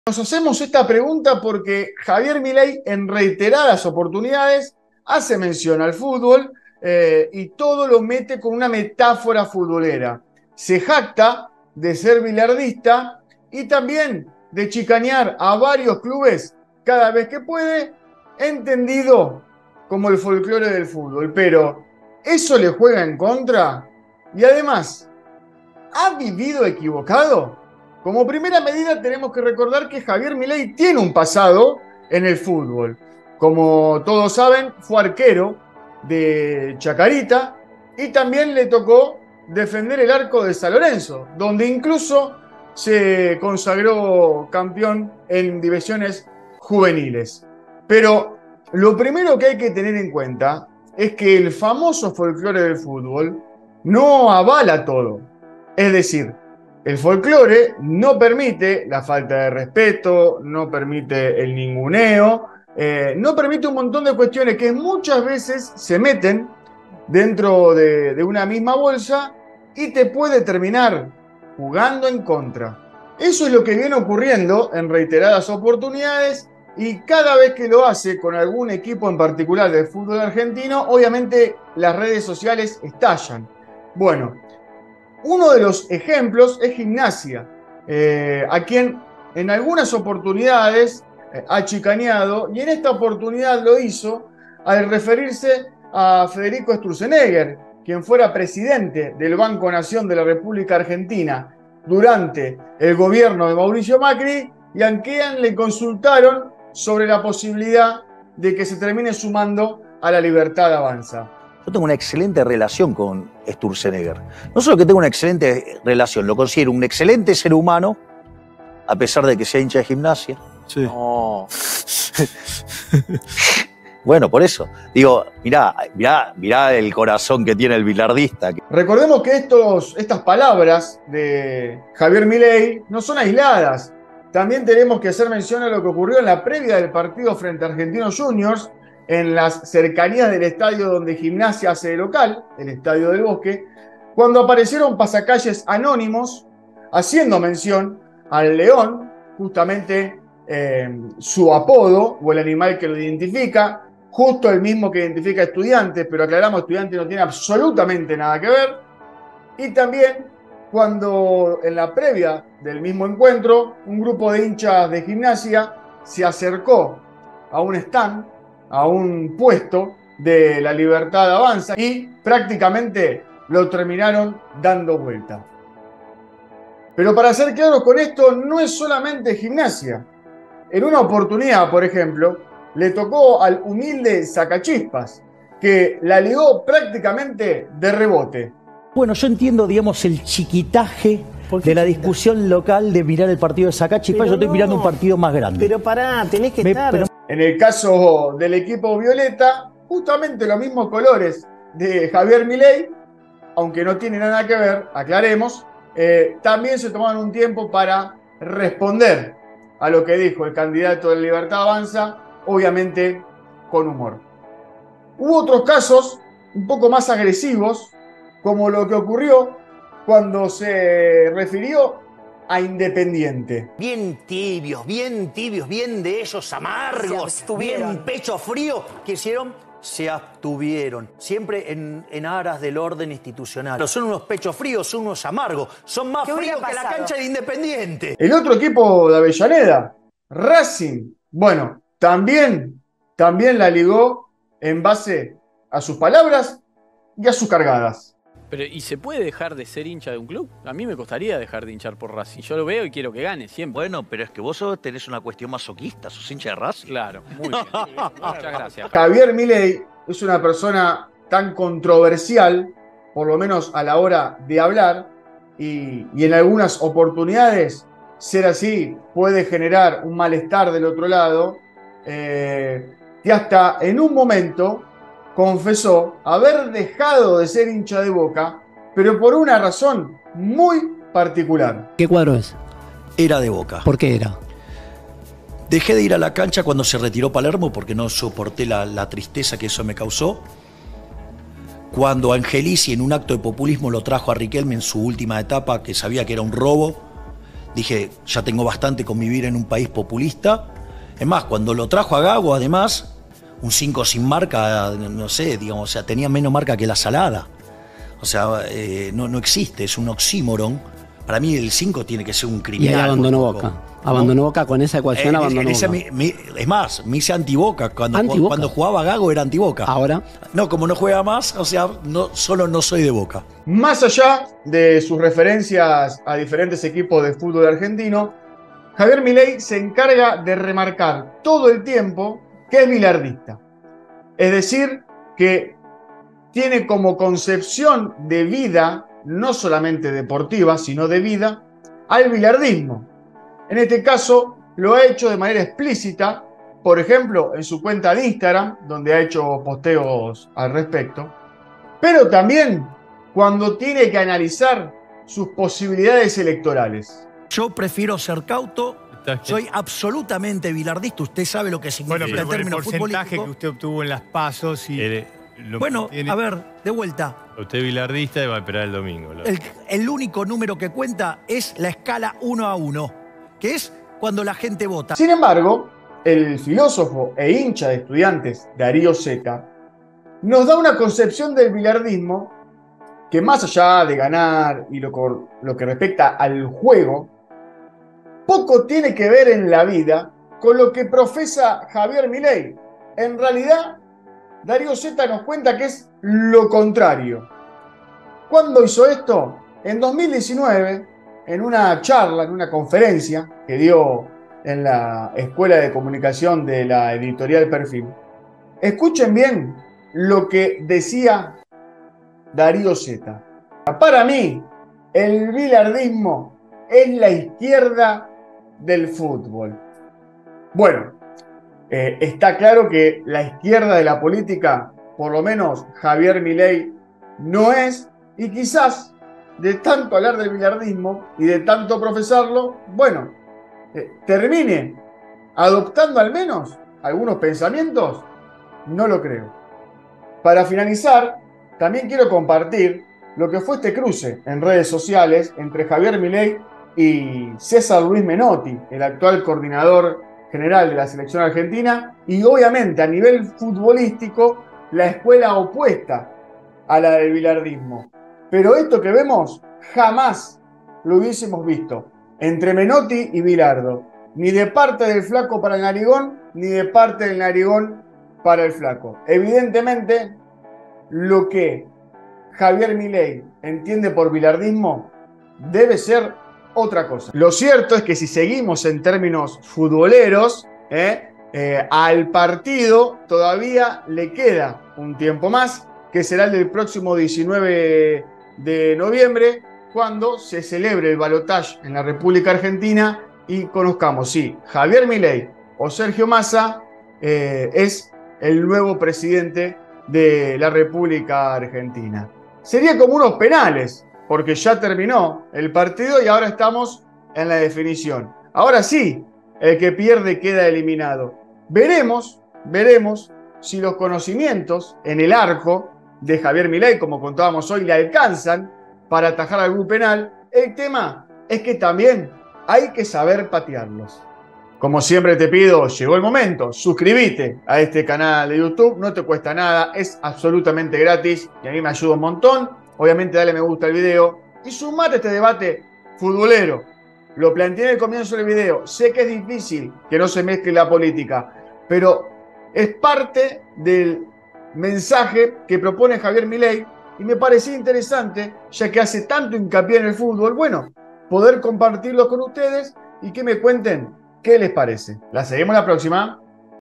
👀 JAVIER MILEI: entre las chicanas futboleras y el BILARDISMO ⚽
Recordemos que Javier Milei tiene un pasado ligado al fútbol por haberlo practicado, siendo arquero tanto de Chacarita como de San Lorenzo. Pero lo primero hay que decir que el folklore del fútbol no es para todos, y hay veces que terminas quedando en off side por no saber moverte en ese mundo. Al partido le queda la definición por penales del domingo 19 de noviembre y habrá que ver si apela a su experiencia como arquero para atajarlos, pero también hay que saber patearlos